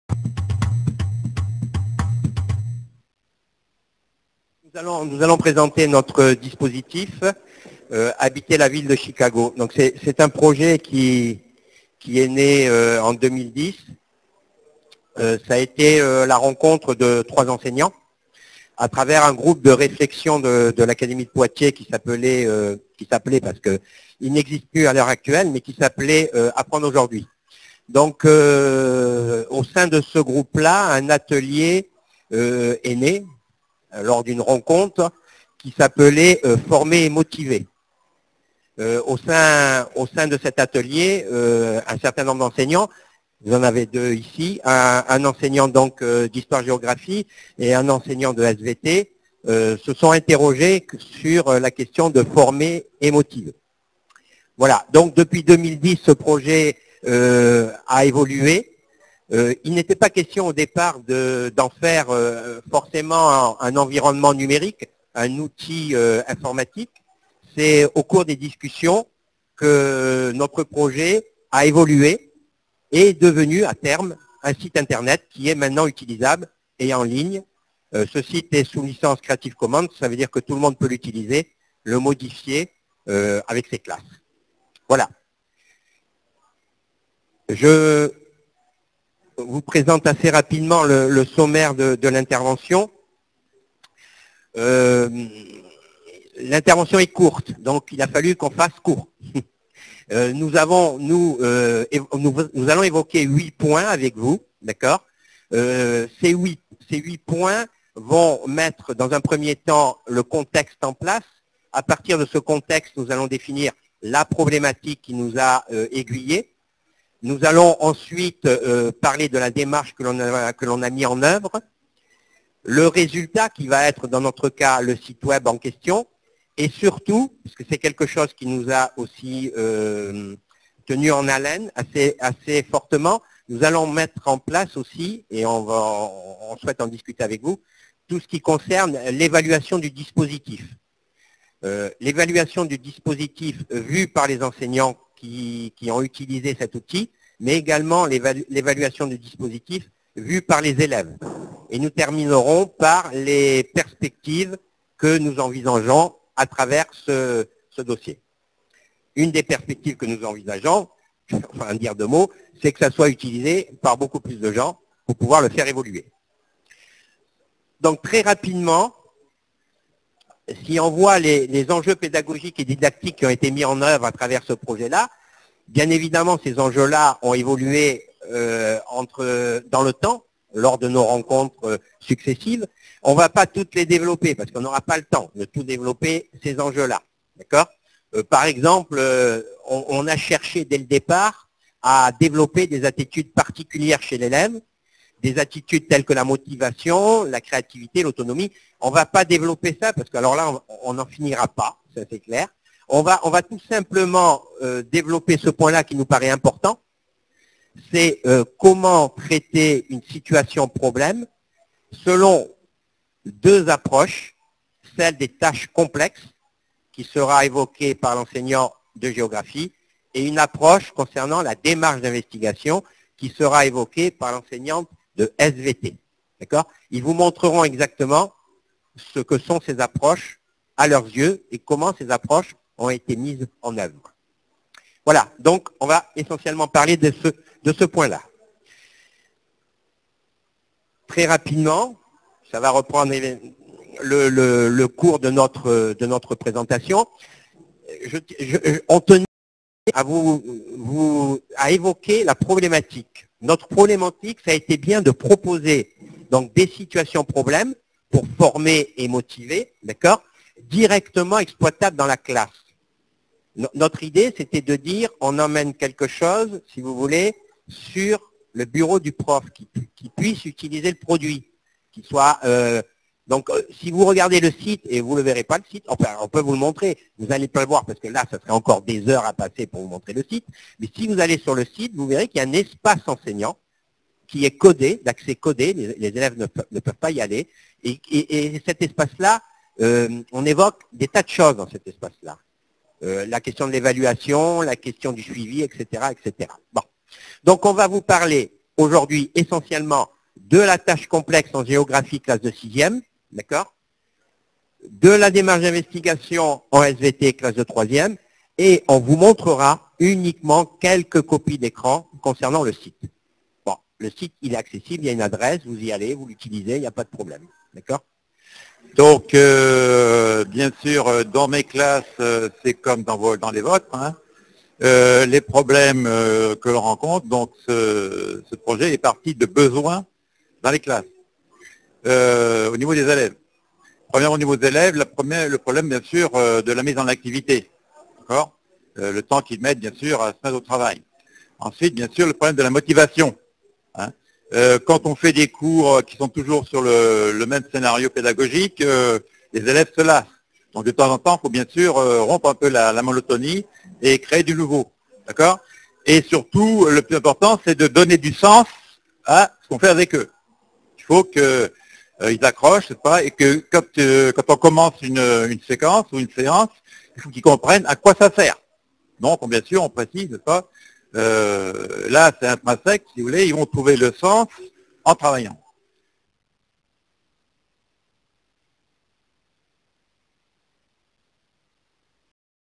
Paris UNESCO 13 mars 2013 Concours de l'innovation pédagogique
Un environnement numérique pour l'apprentissage : Habiter la ville de Chicago.
Un enquête policière associant deux matières, la géographie urbaine et la SVT.
Apprendre en cherchant et trouver la solution en croisant des informations.